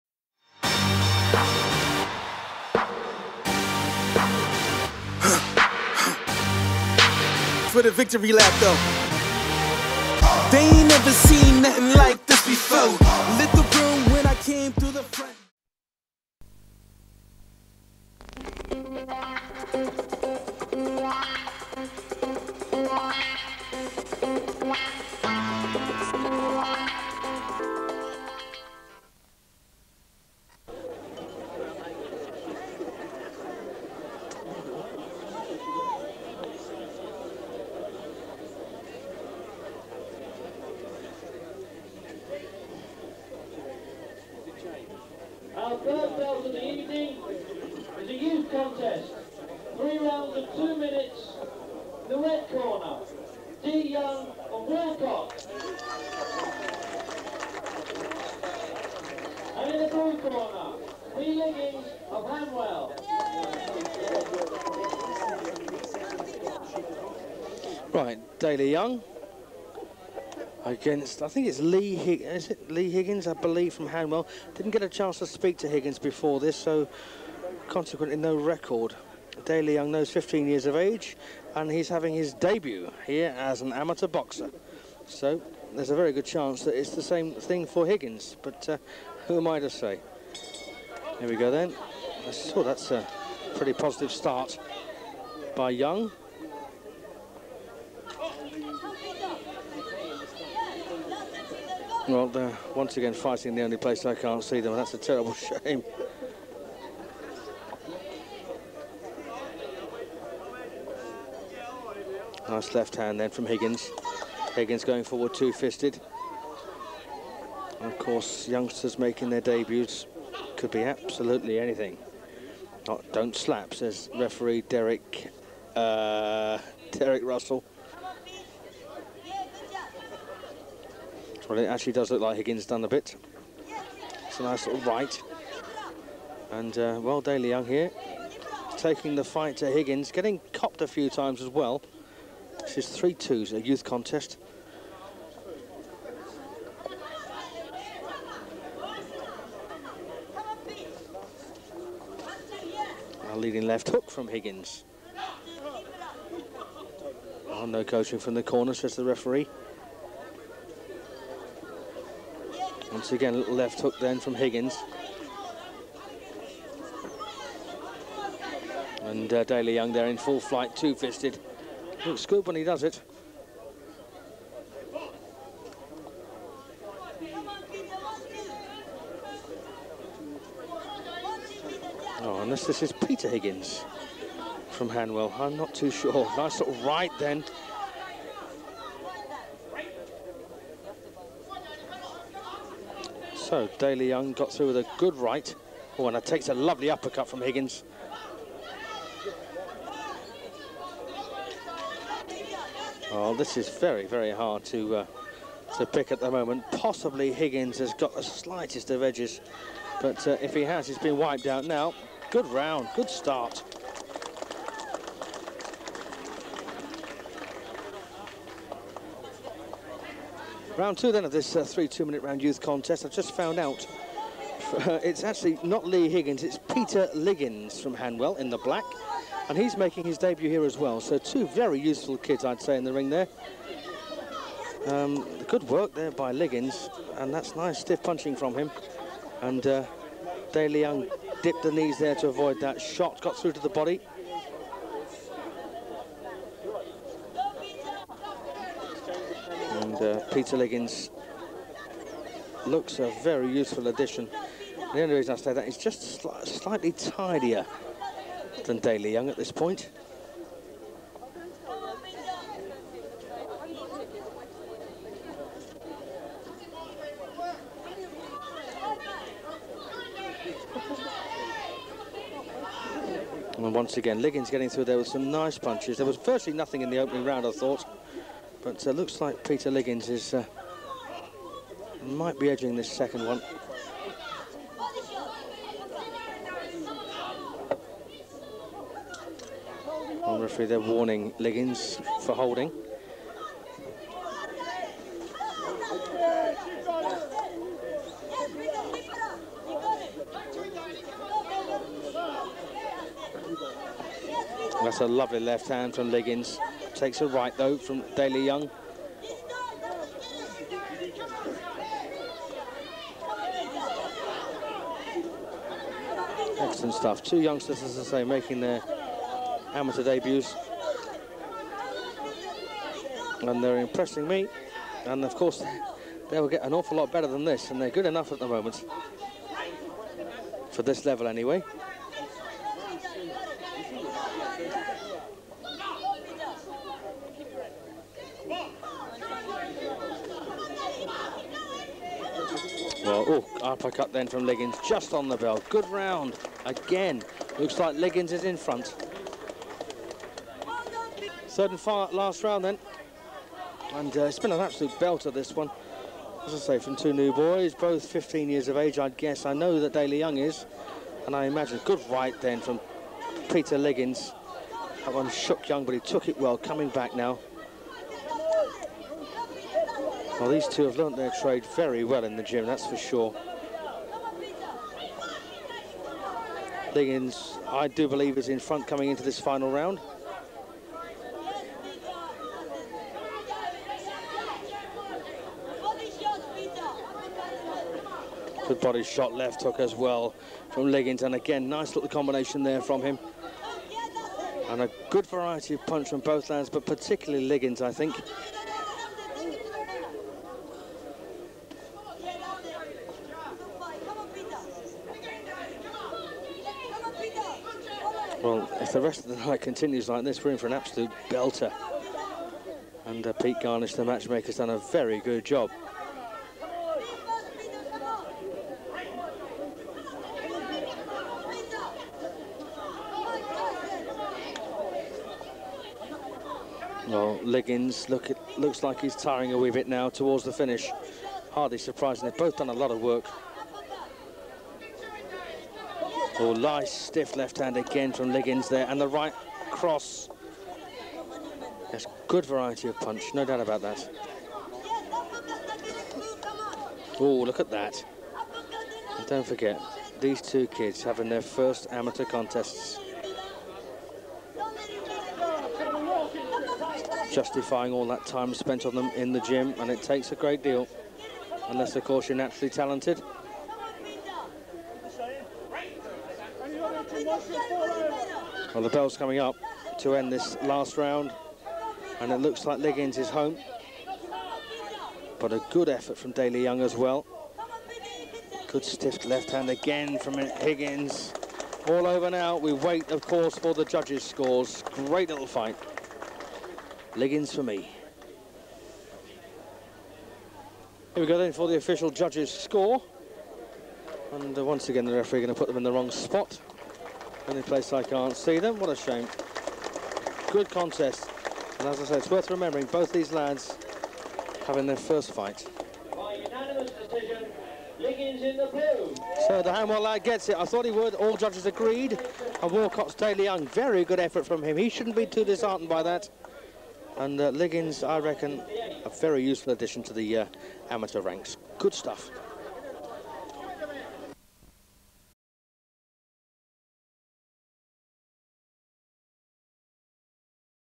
Boxing Highlights (Pete Liggins Debut 1996)
This is Box Clever's Head Trainer, Pete Liggins, debut back on the 12th February 1996. He would go on to become a National Boxing Championship Silver Medalist.

Fight card: P. Liggins vs D. Young